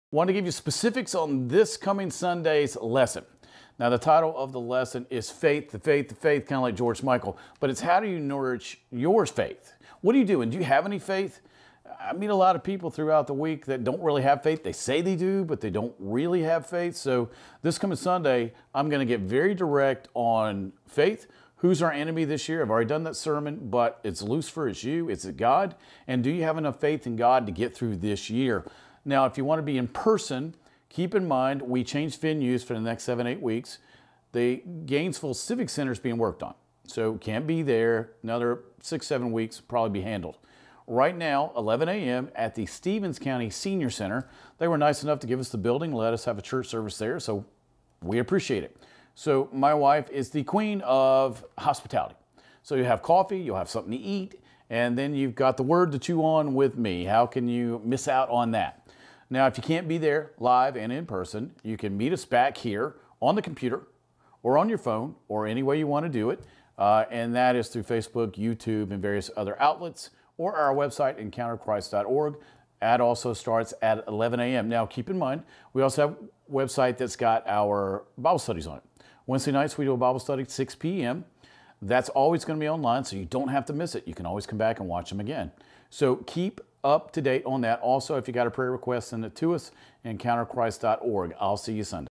1-9-22 Sermon Preview "How to Nourish Your Faith"
How often do you spend time in the Word of God? Do you greet hardship with anxiety and negativity? Tune in Sunday at 11AM to hear Pastor Scot Ferrell discuss how vital it is to nourish your faith.